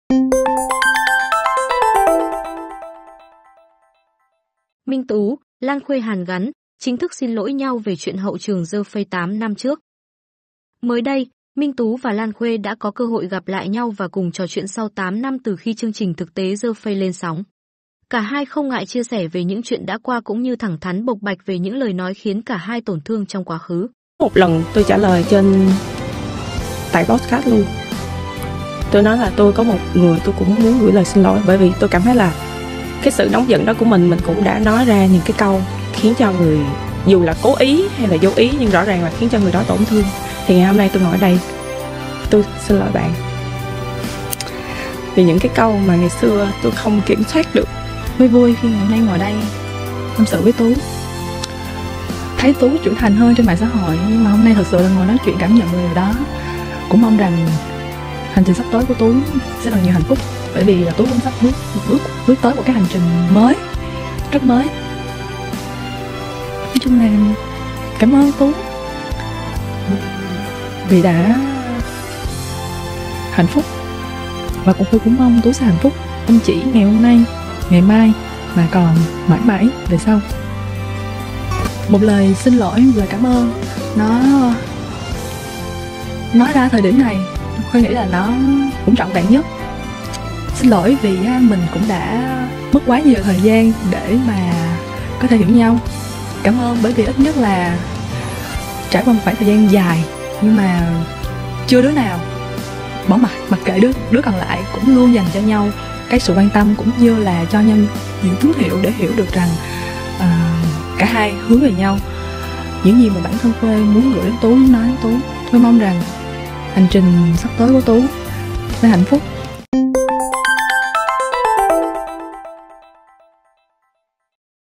Minh Tú - Lan Khuê hàn gắn, chính thức xin lỗi nhau về chuyện "hậu trường" The Face 8 năm trước
HHT | Mới đây, Minh Tú và Lan Khuê đã có cơ hội gặp lại nhau và cùng trò chuyện sau 8 năm từ khi "The Face" lên sóng. Cả hai không ngại chia sẻ hết lòng mình cũng như thẳng thắn nói về những lời nói từng tổn thương nhau trong quá khứ.

Nguồn: Minh Tú, Lan Khuê